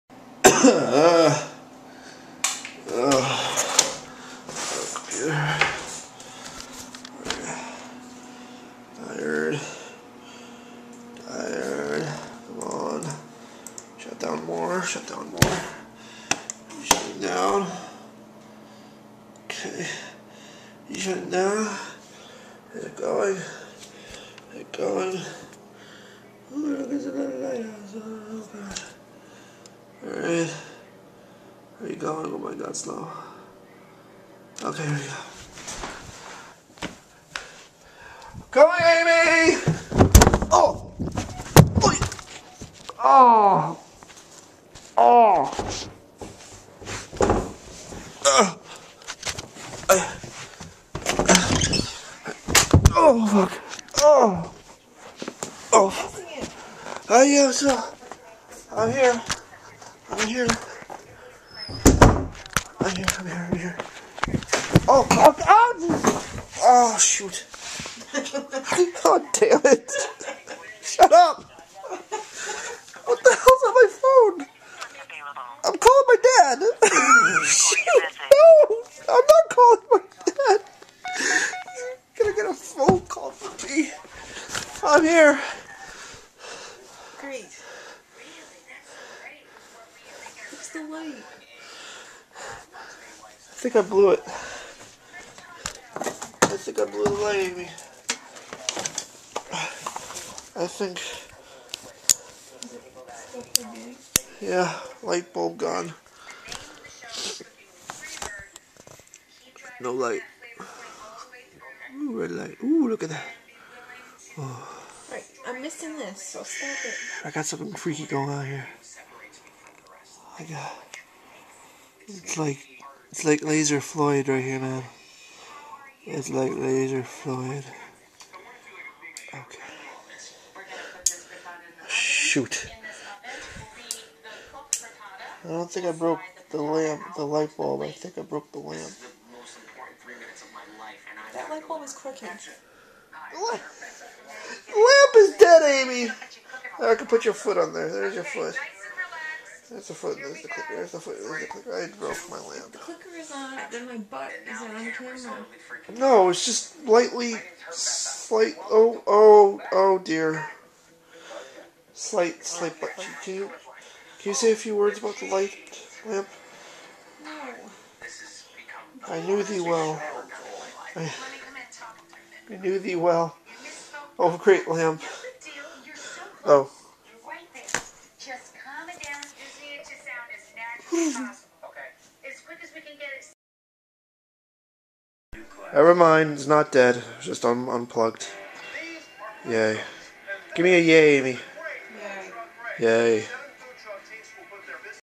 73 - I'm Super Tired (& Love Lamp)
I was tired! ... Really tired! ... And in a silly mood. ... Unfortunately it almost cost me my lamp! ... Luckily, it was ok. ... ... ... yay lol